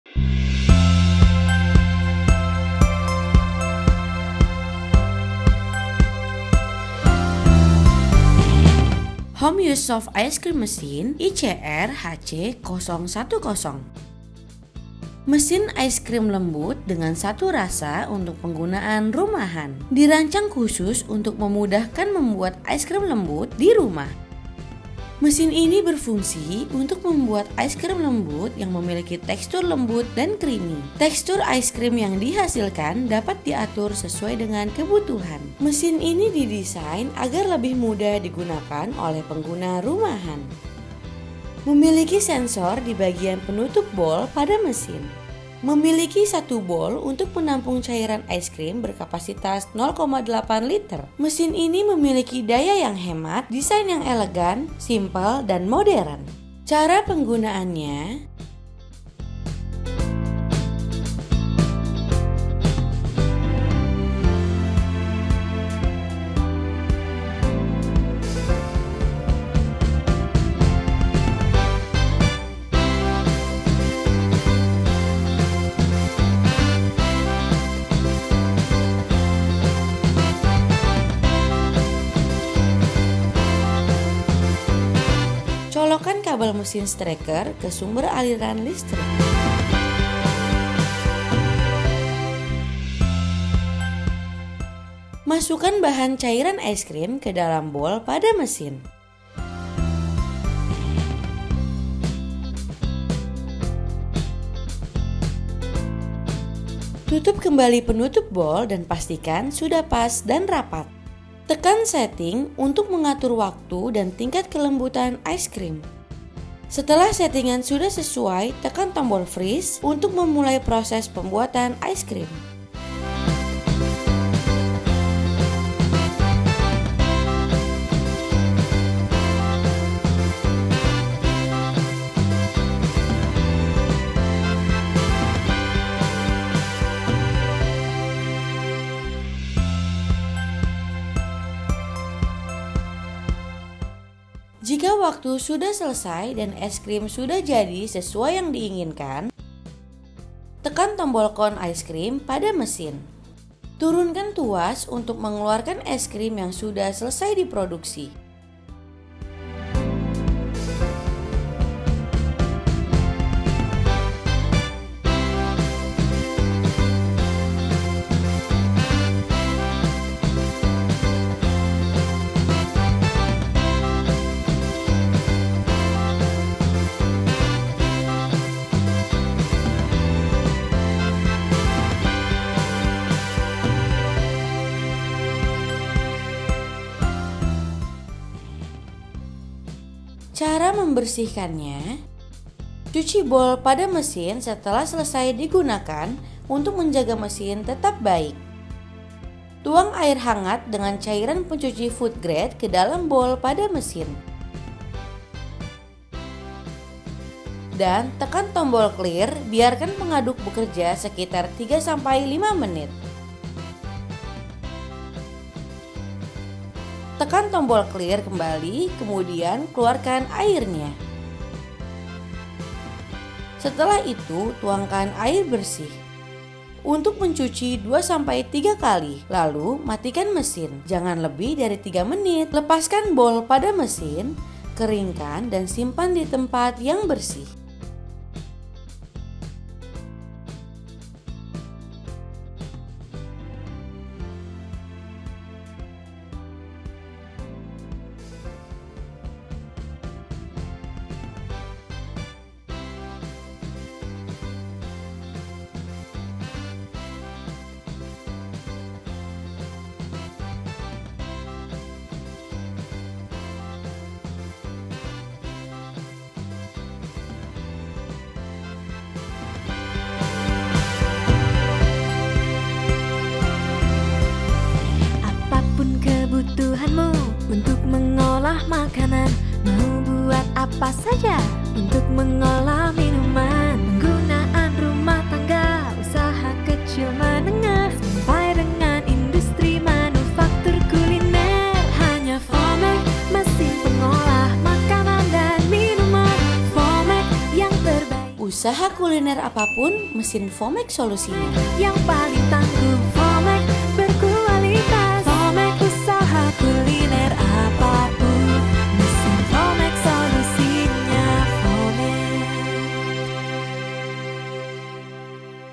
KEMUDAHAN MEMBUAT ES KRIM DARI RUMAH | ES KRIM Ice Cream Maker | ICR-HC010
Yuk sama-sama kita akan lihat keunggulan mesin pembuat es krim yang menjadi jawaban bagi bisnis kecil dan pengunaan pribadi! Temukan kemudahan dan kreativitas dalam setiap es krim yang dihasilkan. Mesin ini tidak hanya memberikan rasa lezat, tetapi juga membuka peluang bisnis yang menjanjikan. Saksikan bagaimana alat ini dapat memberikan keunggulan untuk meningkatkan produksi dan kualitas es krim Anda. Jangan lewatkan kesempatan untuk memulai bisnis es krim Anda di rumah!